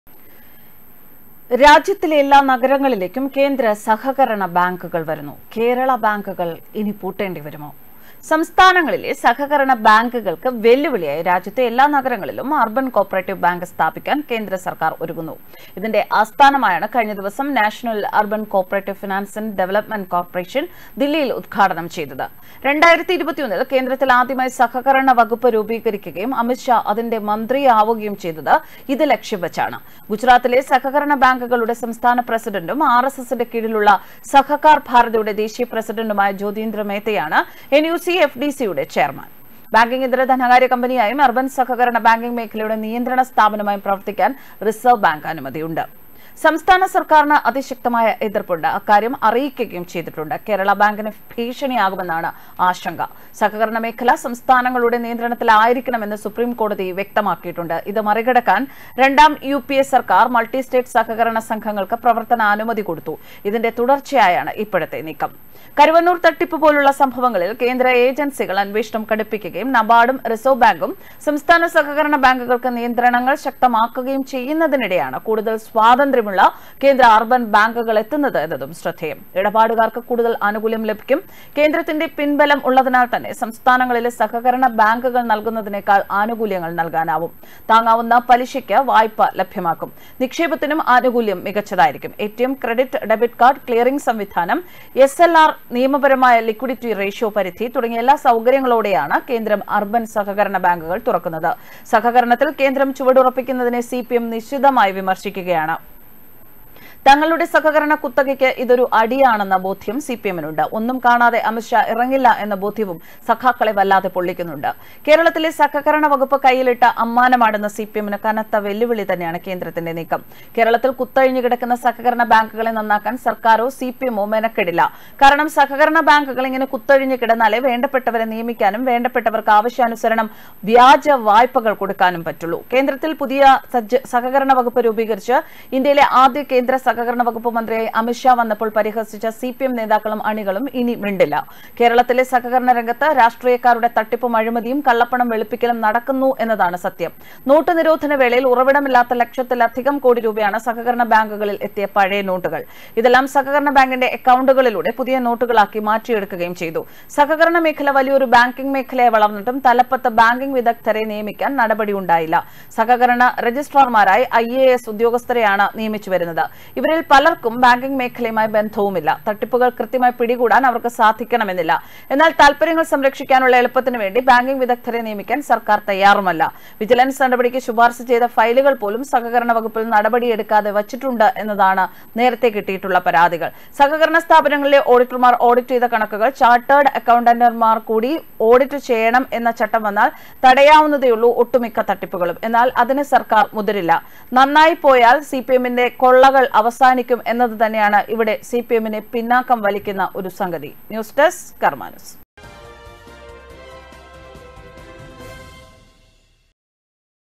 കേന്ദ്രസഹകരണബാങ്കുകൾ വരുന്നു ;കേരളബാങ്കുകൾക്കു ഭീഷണി
സഖാക്കന്മാരുടെ നെഞ്ചിടിപ്പിക്കാൻ കേന്ദ്രസഹകരണബാങ്കുകൾ വരുന്നു ,കേരളബാങ്കുകൾക്കു പൂട്ട്